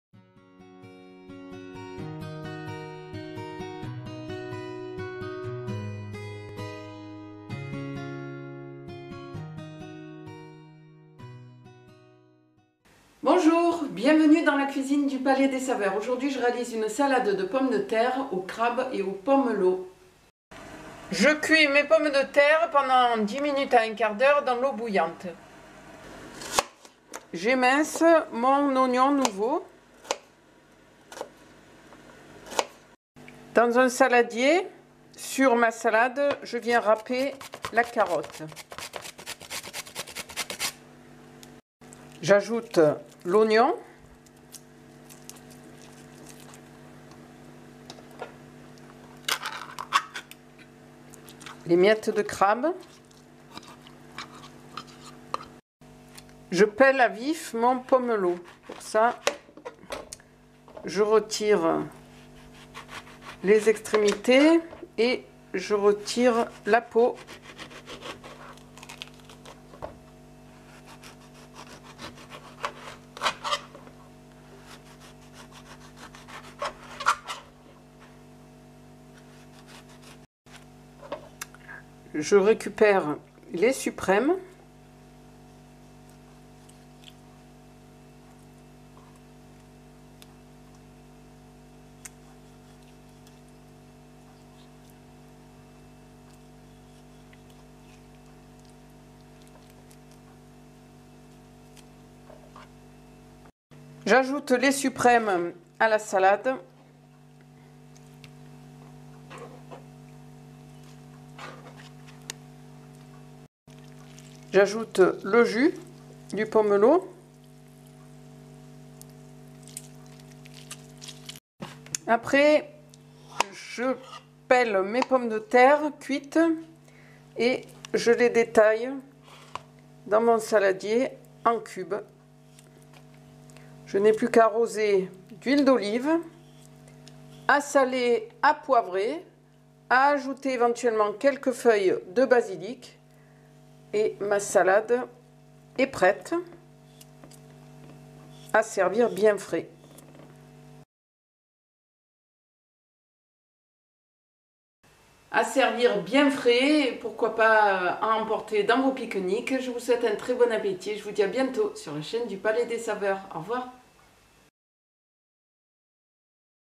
Recette de salade de pommes de terre au crabe et pomelo
Une belle idée du magazine "Gourmand", pour remplir ma lunch box.